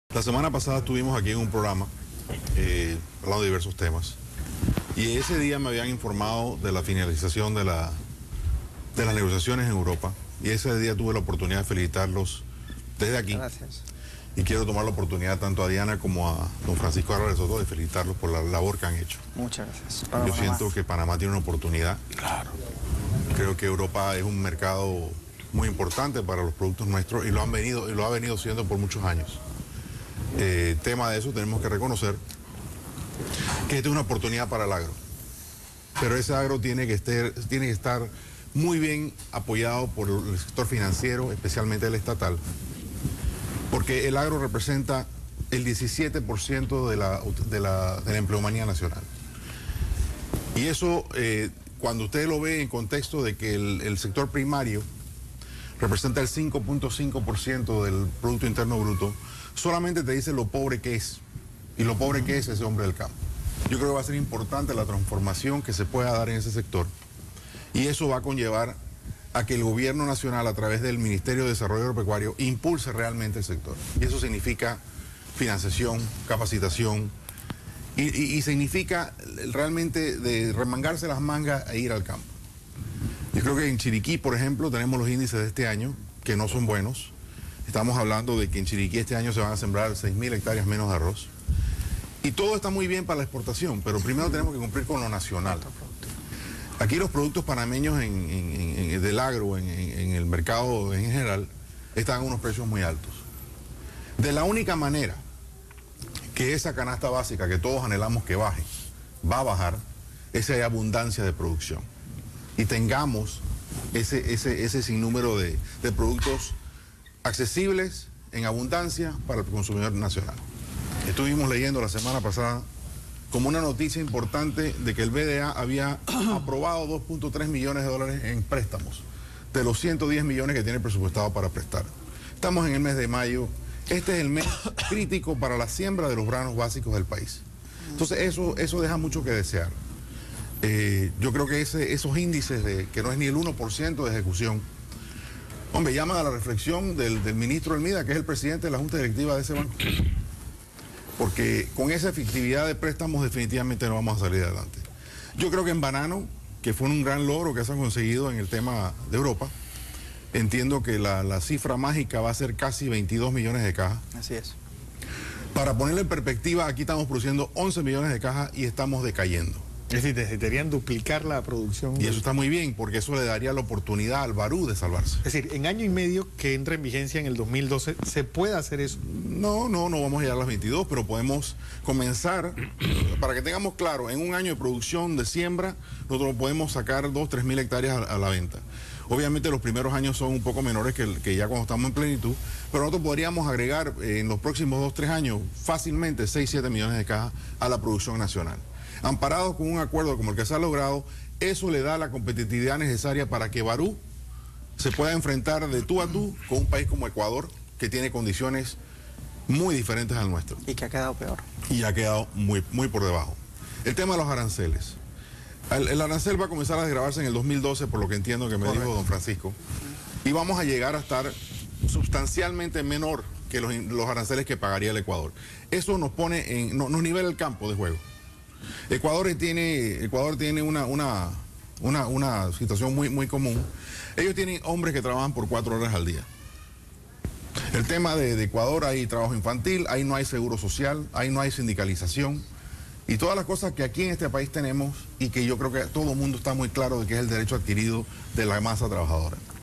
BOLO FLORES (HABLANDO DE LAS VENTAJAS DEL TLC PARA PANAMA)
Hablando de las repercusiones con la firma del TLC con la Union Europea